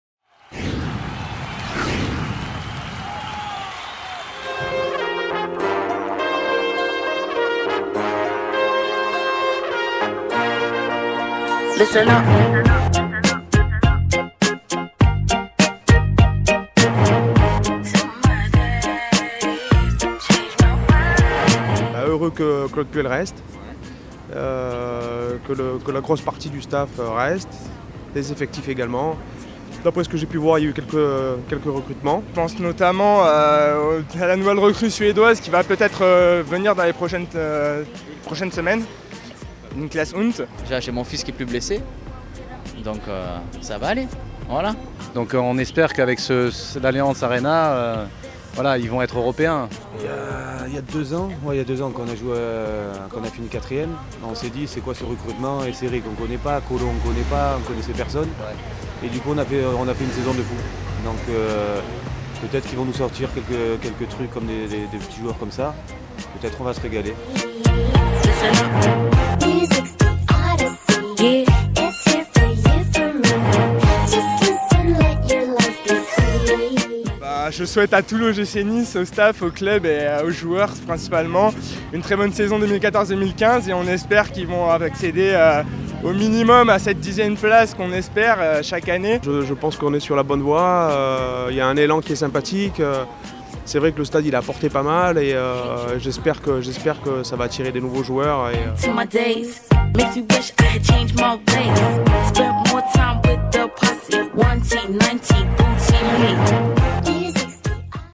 Pendant ce temps-là, à Nice
Alors que la Coupe du Monde bat son plein, Nice commence à se projeter sur la saison 2014-15. La campagne "Cultivons notre différence" fleurit un peu partout en ville. Reportage.